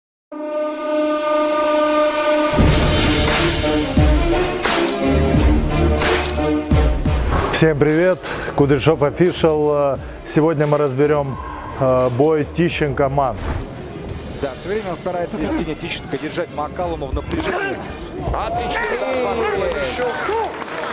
Евгений Тищенко VS Артур Манн (Анонс, скоро выход полного видео на канале)
В воскресенье 11 декабря на арене «Академии единоборств РМК» в Екатеринбурге, Россия 10-раундовый бой Евгения Тищенко и Артур Манн